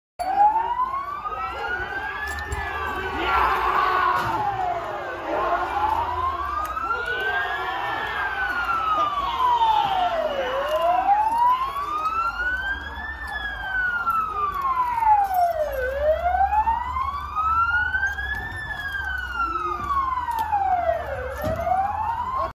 Gates set alight at the peace line between between the Shankill Road and Lanark North Belfast
Gates set alight at the peace line between between the Shankill Road and Lanark way interface in North Belfast. Crowds of a few hundred on each side throwing petrol bombs over in both directions.